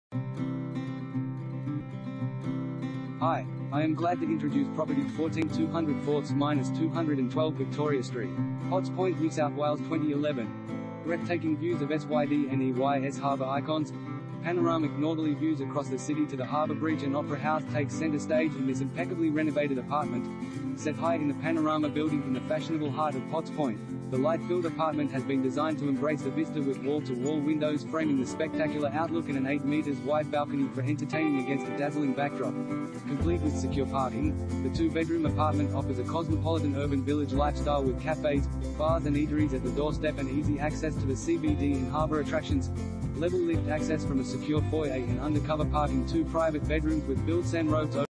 14/204-212 Victoria Street, Potts Point NSW 2011 | AuPropertyReport.Com
14/204-212 Victoria Street, Potts Point NSW 2011
Property Type: apartment
Bedrooms: 2
Bathrooms: 2
Car Space: 1
BREATHTAKING VIEWS OF SYDNEY'S HARBOUR ICONS